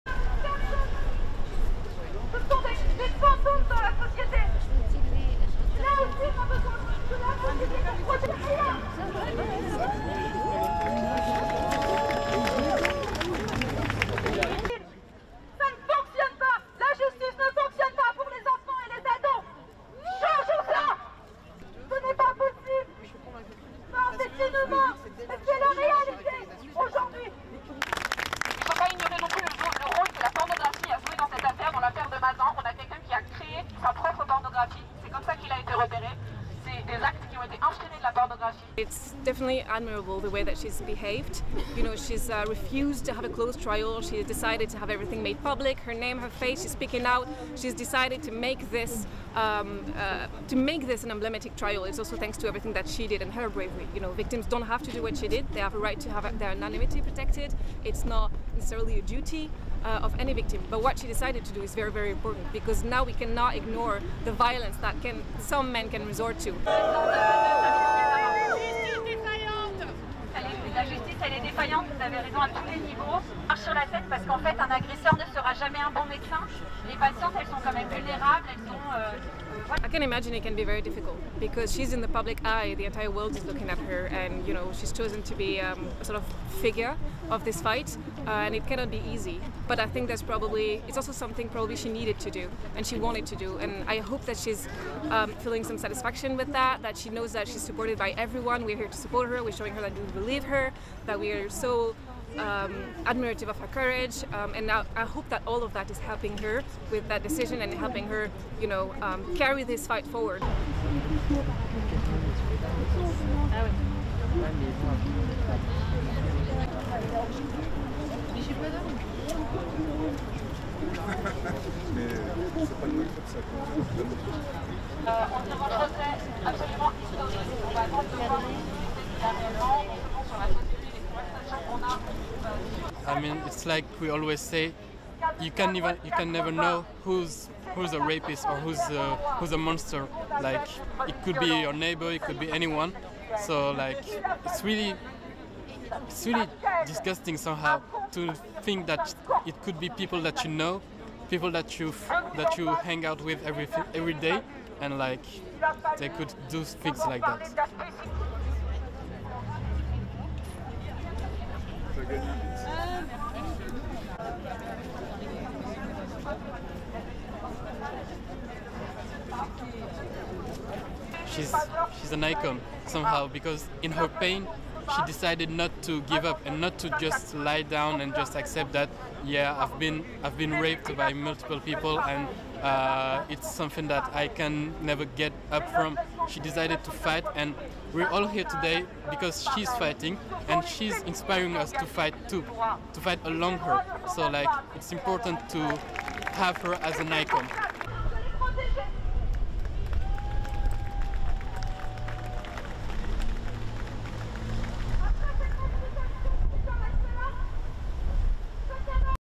Protesters take to streets of Paris against sexual violence in light of Pelicot court case
RESTRICTION SUMMARY:

ASSOCIATED PRESS
Paris - 19 October 2024
1. Protesters outside Paris criminal court house
2. Woman holding sign reading (English) “Shame must change sides”
3. Woman holding sign reading (French) “Shame must change sides”
3. Protesters holding sign reading (French) “We are all Gisele. Are you all Dominique?”
4. Mid of protest
5. SOUNDBITE (English) Elsa Labouret, spokesperson for the women’s group “Osez le féminisme!” (Dare to be feminist!):
“It's definitely admirable the way that she's behaved. You know, she's refused to have a closed trial. She has decided to have everything made public. Her name, her face. She’s speaking out. She's decided to make this an emblematic trial. It's also thanks to everything that she did and her bravery. You know, victims don't have to do what she did. They have a right to have their anonymity protected. It's not necessarily a duty of any victim. But what she decided to do is very, very important because now we cannot ignore the violence that some men can resort to."
6. Various of protest
7. SOUNDBITE (English) Elsa Labouret, spokesperson for the women’s group “Osez le féminisme” (Dare to be feminist!):
“I can imagine it can be very difficult. Because she's in the public eye. The entire world is looking at her. And, you know, she's chosen to be a sort of figure of this fight. And it cannot be easy. But I think that's probably, it's also something probably she needed to do, and she wanted to do. And I hope that she's feeling some satisfaction with that, that she knows that she's supported by everyone. We're here to support her. We're showing her that we believe her, that we are so admirative of her courage. And now, I hope that all of that is helping her with that decision and helping her, you know, carry this fight forward."
8. Various of Feminist group protesting outside Paris tribunal
9. Man holding sign and drawing reading (French) “Stop the denial”
10. SOUNDBITE (English) Khalil Ndiaye, protester:
"I mean, it's like we always say, you can never know who is a rapist or who is a monster. Like, it could be your neighbour, it could be anyone. So, like, it's really disgusting somehow to think that it could be people that you know, people that you hang out with every day and, like, they could do things like that."
11. Protesters holing signs
12. Sign reading (French) “Witnesses to sexual violence against minors, keeping quiet is being an accomplice”
13. SOUNDBITE (English) Khalil Ndiaye, protester:
"She's an icon somehow, because in her pain, she decided not to give up and not to just lie down and just accept that ‘Yeah, I've been raped by multiple people and it's something that I can never get up from’. She decided to fight. And we're all here today because she's fighting and she's inspiring us to fight, too, to fight along(side) her. So, like, it's important to have her as an icon."
14. Protest
STORYLINE:
Women and men demonstrated together Saturday in Paris and other French cities in support of Gisèle Pélicot and against sexual violence.

Pélicot has become a symbol for the feminist cause following the harrowing trial of her ex-husband and dozens of other men, prosecuted for raping the 71 year-old while she was deliberately drugged and unconscious.

Demonstrations outside Paris' criminal court, in the southeastern city of Lyon and elsewhere underscored how Pélicot's courage in speaking out about her ordeal is inspiring people in France and beyond.

"Victims don’t have to do what she did. They have a right to have their anonymity protected," Labouret added.